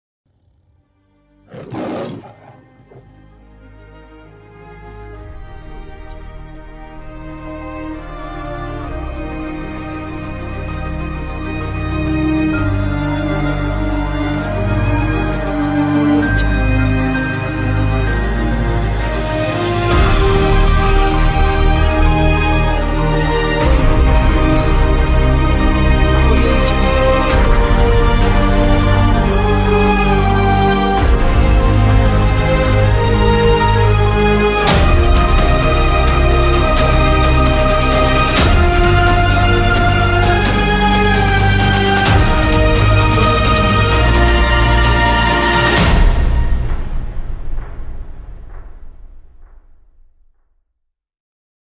What has Valentine's Day got to do with project intelligence? Watch the movie 🎬trailer now.
IBIS is a turnkey solution for digitizing the project sales process and addresses the major pitfalls of conventional CRM systems in terms of implementation costs and success in generating a return on investment.